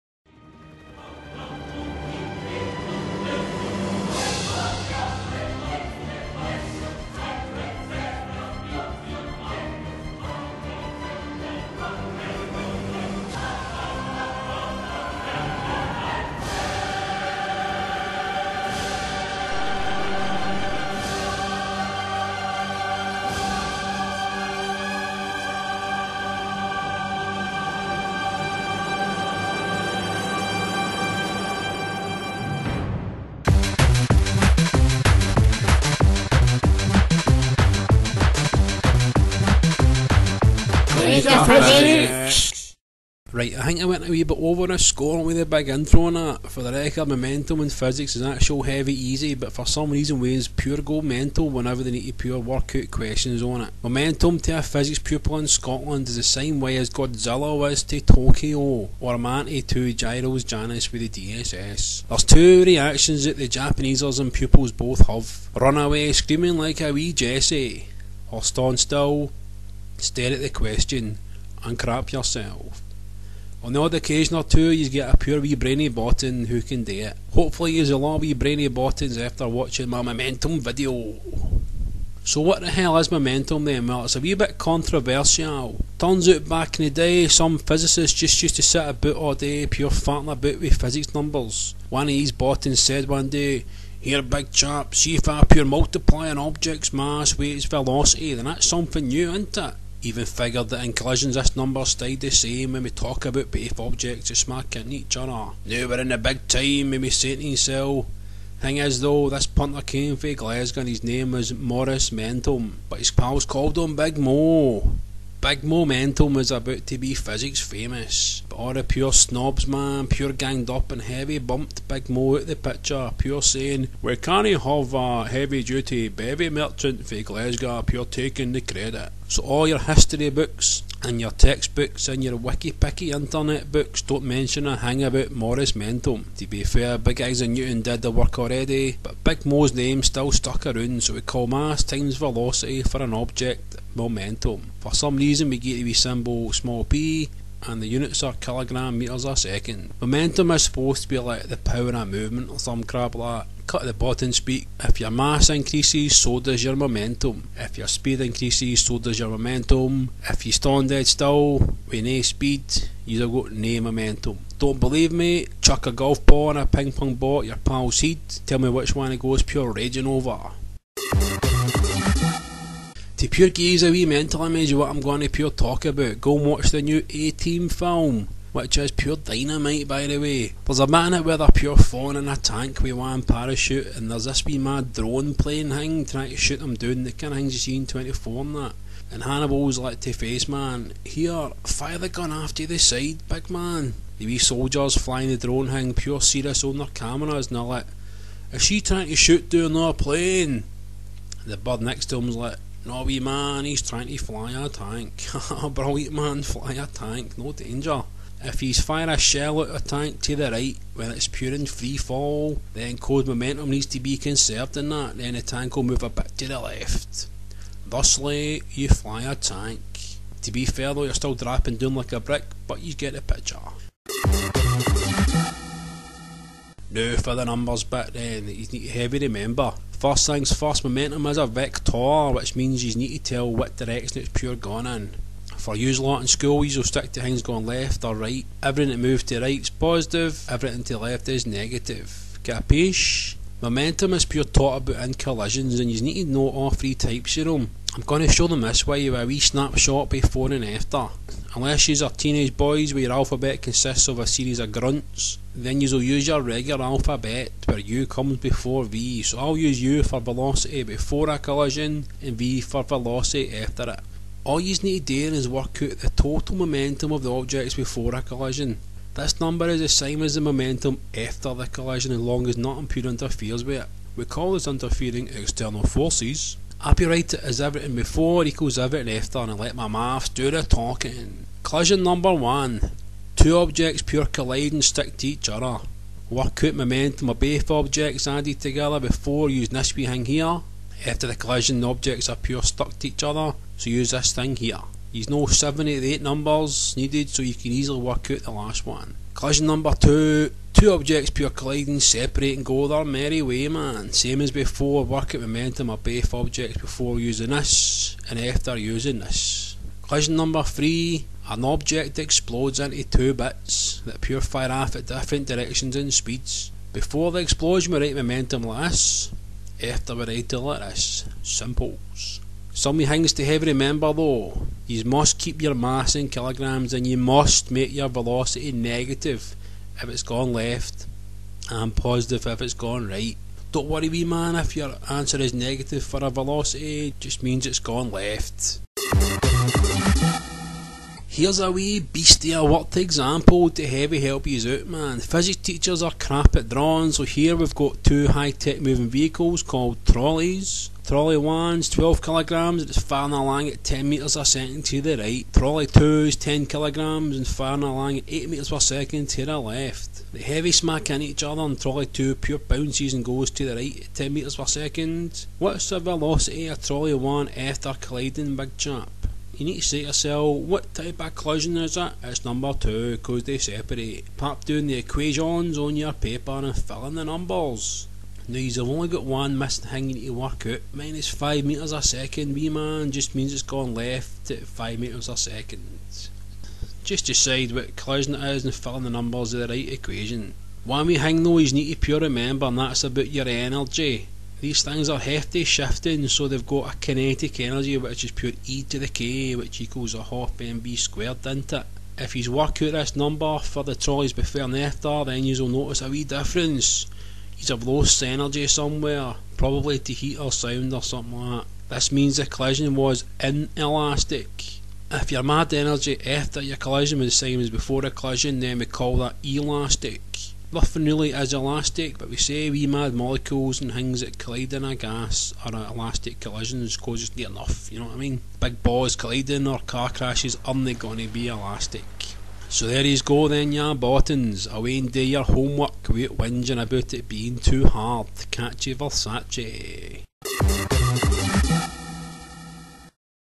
Glasgow Physics 9. Momentum
Valdo shows just how simple momentum can be.